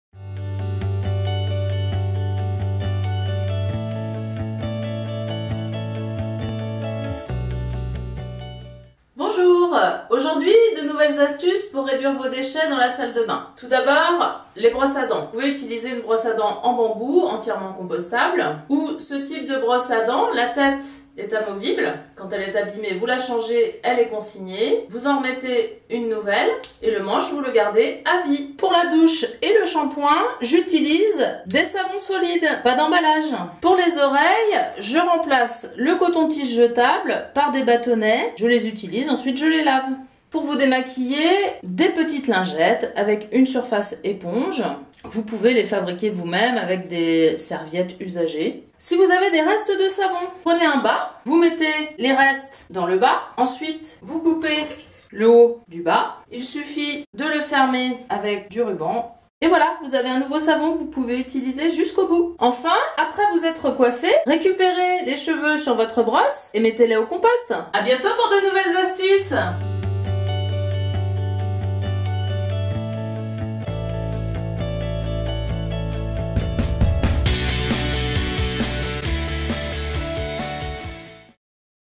Les tutos de Lisbeth #9 : éco-gestes dans la salle de bain
Avec ce tutoriel sur les éco-gestes dans la salle de bain, Lisbeth, animatrice Prévention des Déchets à la Communauté de Communes Châteaubriant Derval, vous livre ses trucs et astuces !
Vous pouvez aussi retrouver sur notre chaine le tuto de Lisbeth #8 sur les éco-gestes dans la cuisine.